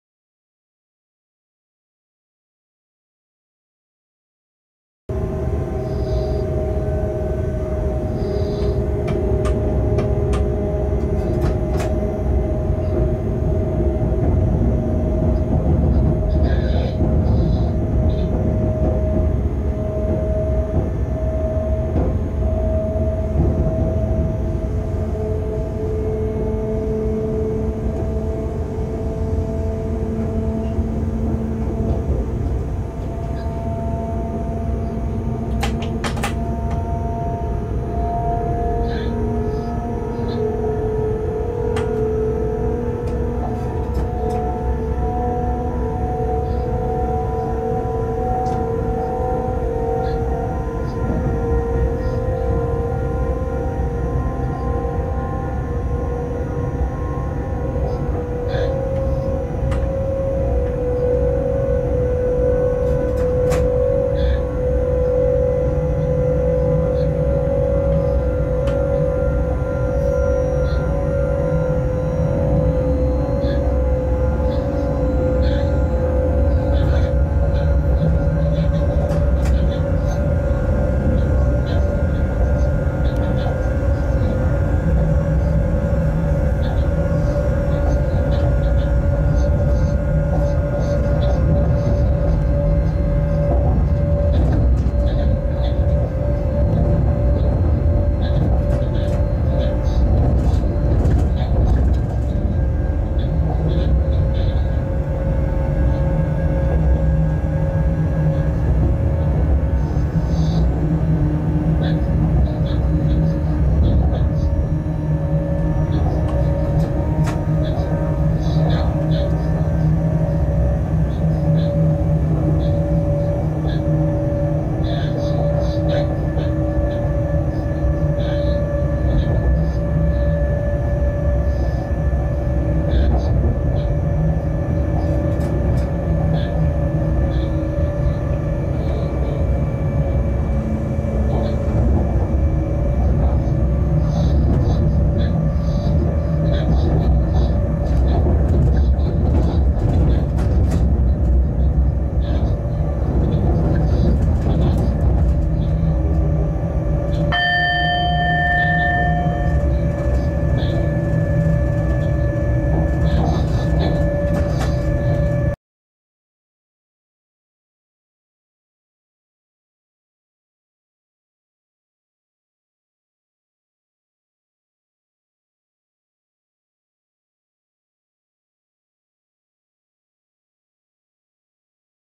Train cab ride; Almelo (via track 205) (20240515)
Cab view of a freight train ride running through Almelo via track (20)5.

* this is part of a ride from Lehrte to Tilburg Industrieterrein. In the other version the ride runs via track 207.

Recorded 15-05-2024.